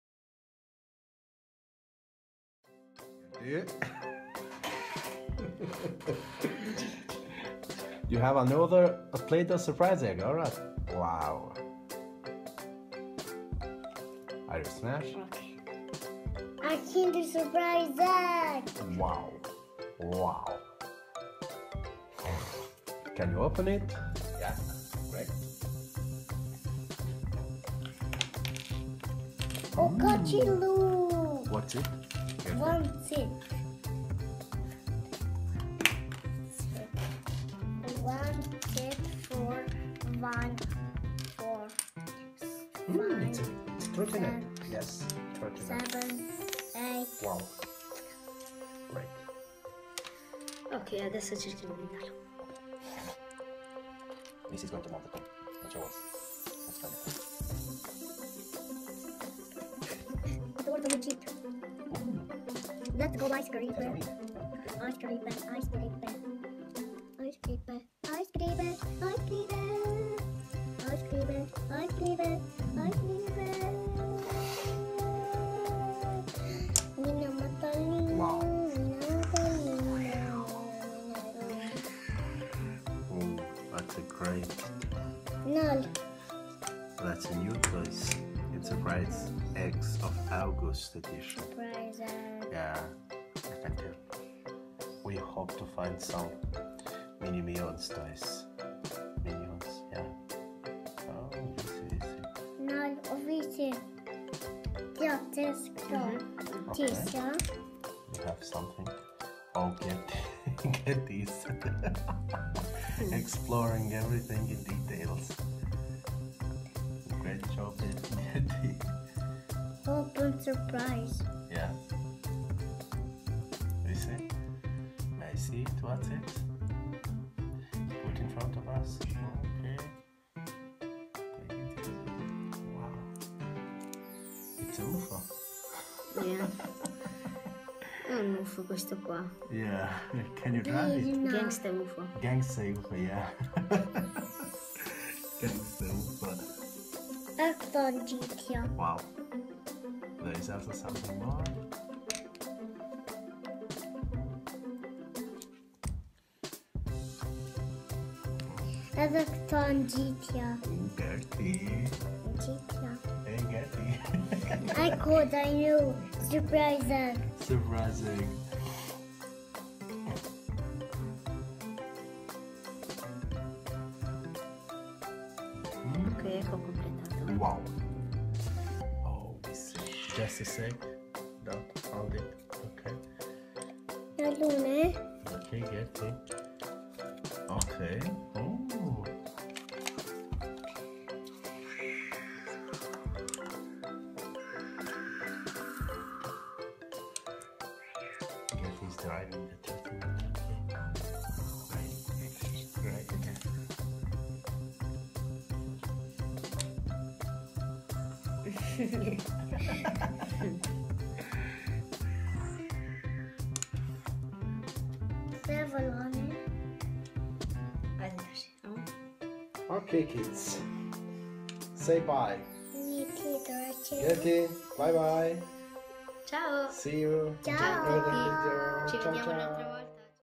Play Doh Trotinette Free Style with Kinder Surprise Egg Toys! Playdough Surprise Eggs GERTIT
Hi Surprise Toys lovers everywhere. My name is Gertit and I'm three years old. I love Play Doh, Kinder Surprise and Kids Games.

Today i present you together with my cousin Elvis some Play Doh Surprise Eggs with Surprise Toys and we are both very happy to show you a lot of Toys to entertain you.